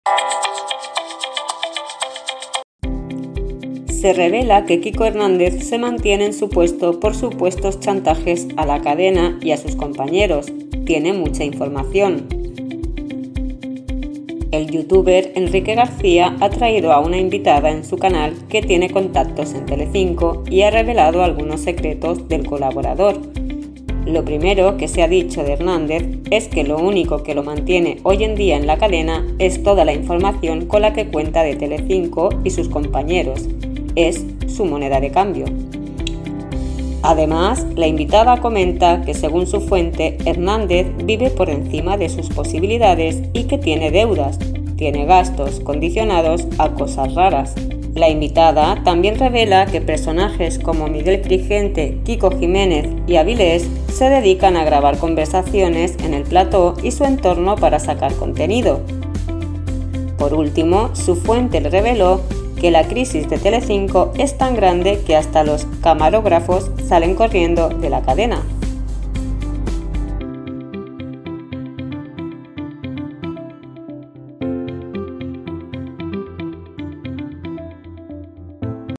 ✋Grave ESCÁNDALO de KIKO HERNÁNDEZ por Sálvame y mediaset
kikohernández es noticia por su grave escándalo, mediaset obligado a tenerlo en plantilla